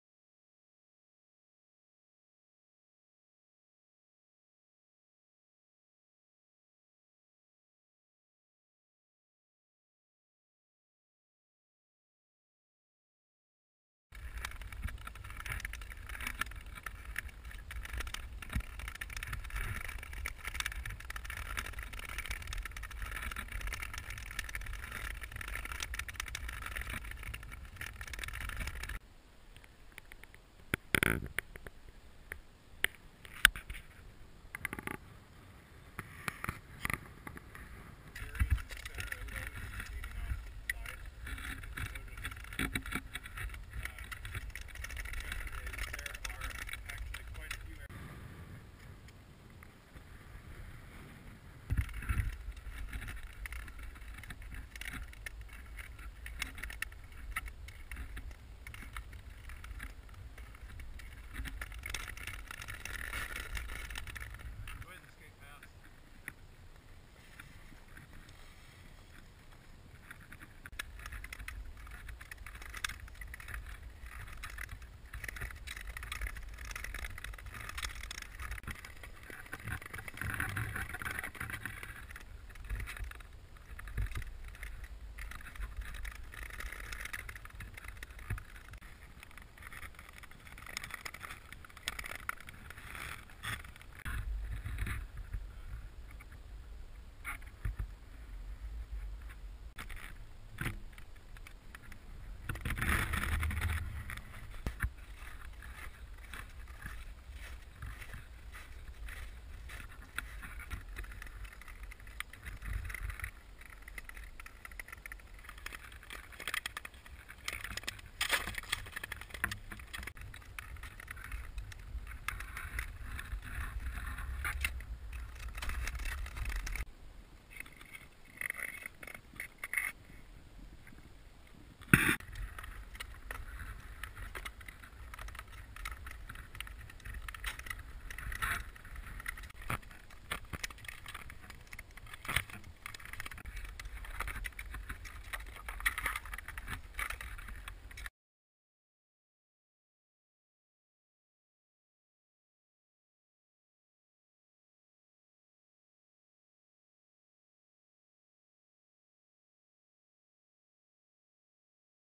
Spring-Skating 2013
Old footage just now getting edited of a trip out the hayflats march 2013.  Last good week of skating that year.  Fun times fine ice and good folks to skate with.